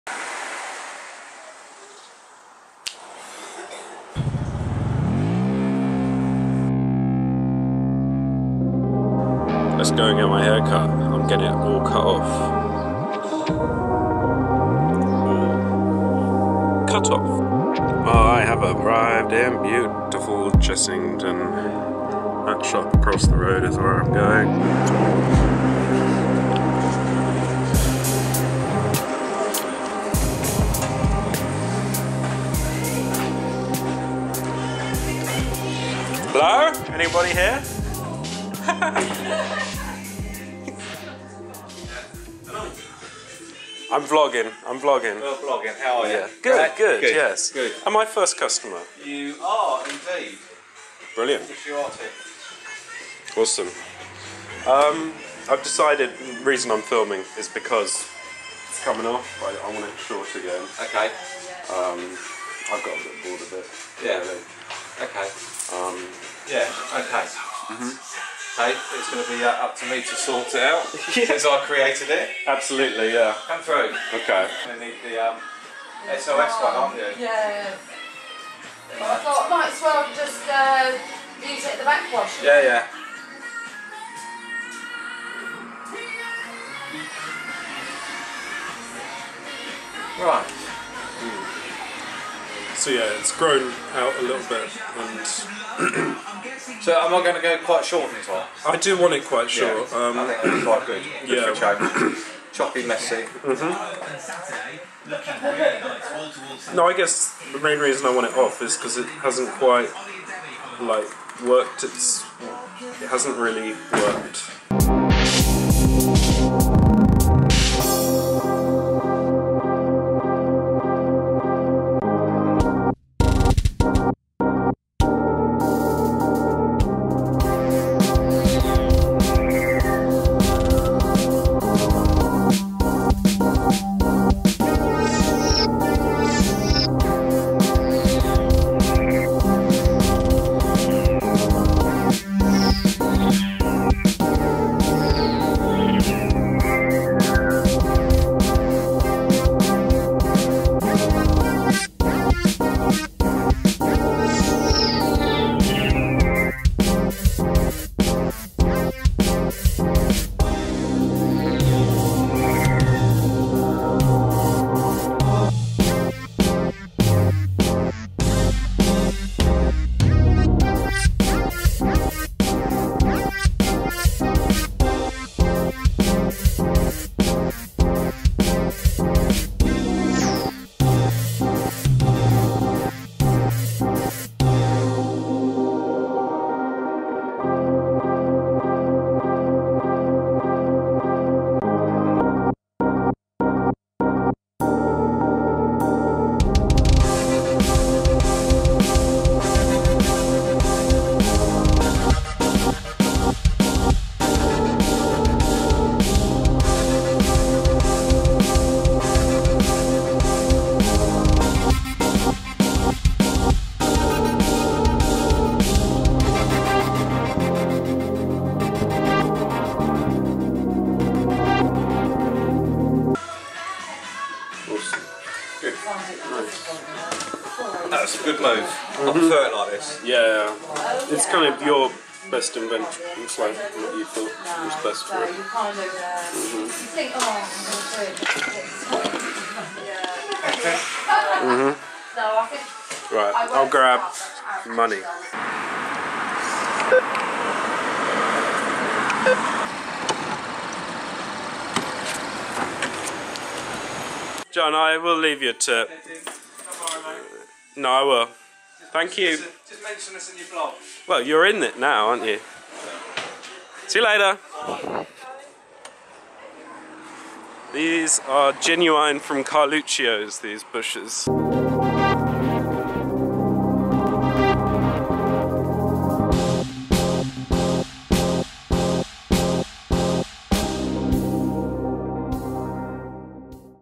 GETTING A HAIRCUT AT JON GARROD
This is Vlog_240915 I decided to get my top knot completely cut off. So here is the resulting video. Big Thanks to Jon for letting me film it!